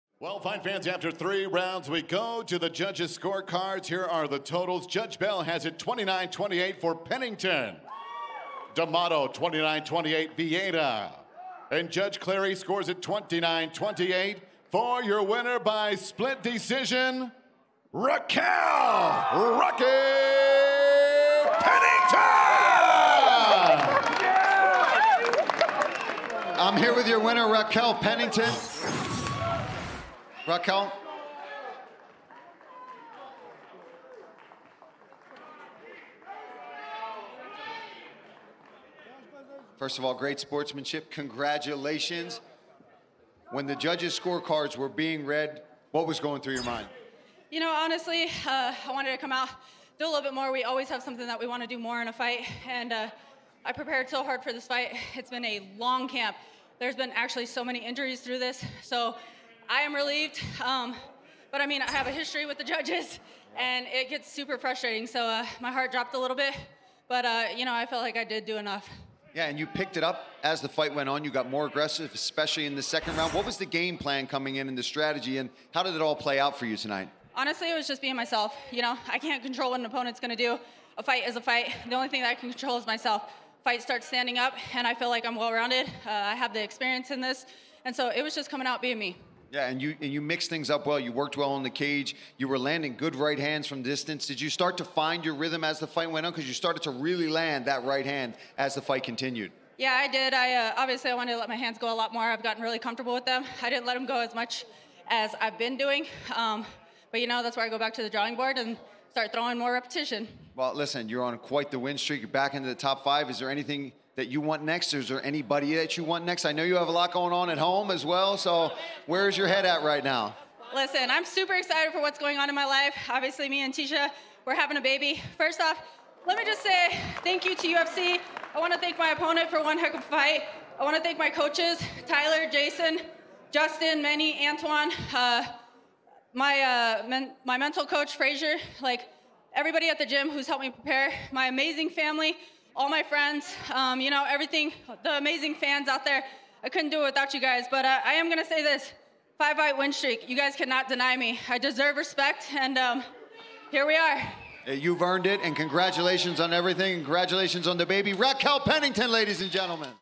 Raquel Pennington Octagon Interview | UFC Vegas 67
UFC bantamweight veteran Raquel Pennington added to her 5-win streak with a victory over #2 ranked Ketlen Vieira at UFC Vegas 67.